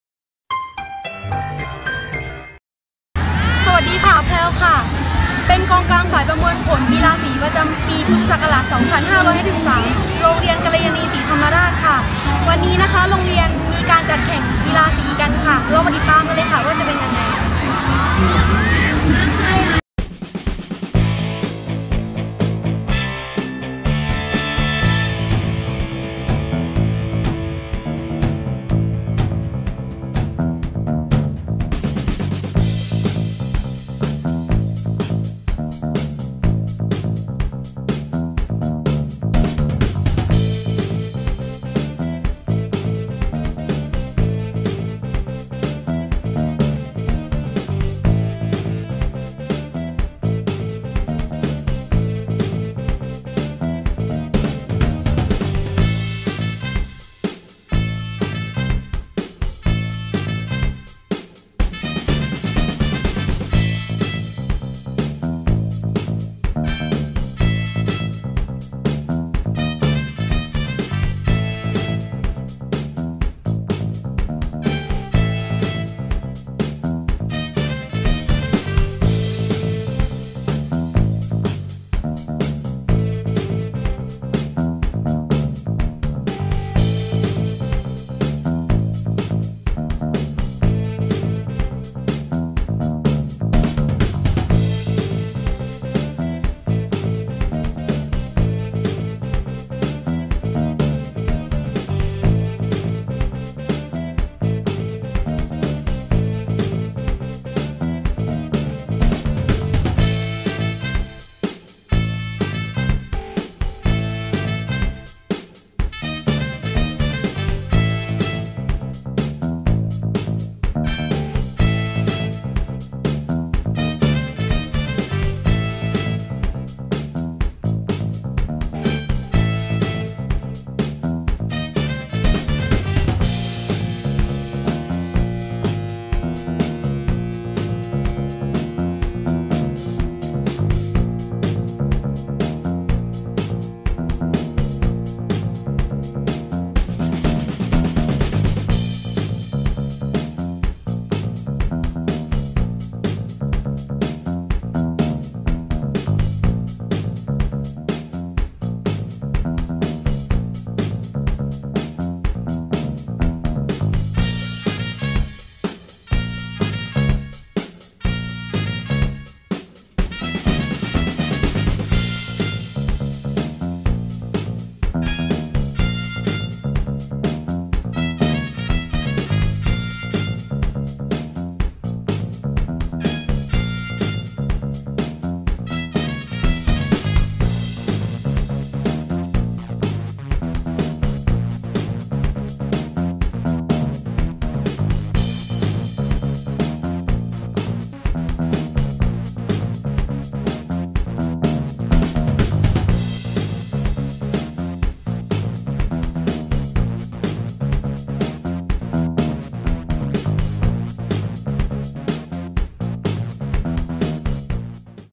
กีฬาสี กัลยาณีฯ 2553
!!!โปรดอ่าน!!!
foreign audience? please see below:

กิจกรรมกีฬาสี ปีการศึกษา 2553
โรงเรียนกัลยาณีศรีธรรมราช
8-9 กรกฎาคม 2553 ณ สนามกีฬาจังหวัดนครศรีธรรมราช

วีดีโอประมวลภาพ (อย่างคร่าวๆ) จากการแข่งขันประจำปี 2553
สำหรับวีดีโอการประชันเชียร์ของแต่ละสี เชิญทางนี้ครับ:

นอกจากนี้ผมยังมีประมวลภาพกิจกรรมโดยรวม (ส่วนใหญ่จะเป็นภาพถ่ายขบวนพาเหรด)
แต่ตอนนี้ผมยังไม่ได้นำรูปขึ้นเว็บไซต์ เนื่องจากติดภารกิจขนานใหญ่
น่าจะเป็นหลังวันที่ 24 กรกฎาคมไปแล้ว จึงจะนำขึ้นเว็บได้ครับ

งานนี้ต้องขอขอบคุณ
1) น้องแพรว ที่ช่วยเป็นพิธีกร (จำเป็น) ให้กับวีดีโอชุดนี้ (หมายเหตุ: เธอไม่ใช่พิธีกรอาชีพ แต่ทำได้ดีทีเดียวครับ)
2) น้องบุ๊ค ที่ไปถ่ายภาพพร้อมกับผมและน้องแพรว
3) น้องอ๋อง ที่ให้ความอนุเคราะห์กล้อง FinePix S2000HD สำหรับบันทึกภาพวีดีโอ
4) คณาจารย์โรงเรียนกัลยาณีศรีธรรมราช ที่อำนวยความสะดวกในการเก็บภาพครั้งนี้

FOREIGN AUDIENCE

Kanlayanee Sport Days, 2010 Academic Year
Kanlayanee Si Thammarat School
Nakhon Si Thammarat, Thailand
July 8-9, 2010 at Nakhon Si Thammarat Sport Stadium

This video is an overview of the activity.

I also have a collection of photo to be uploaded (mostly the parade photo),
but since I'm too busy right now, I'm unable to upload it.
Maybe after July 24, and all will be set.

Special thanks to:
1) N'Praew, for her duty as MC. (Note that she isn't a professional MC, but she've done her part quite well).
2) N'Book, for his duty as the camera assistant.
3) N'Oung, for his FinePix S2000HD.
4) Teachers of Kanlayanee Si Thammarat School.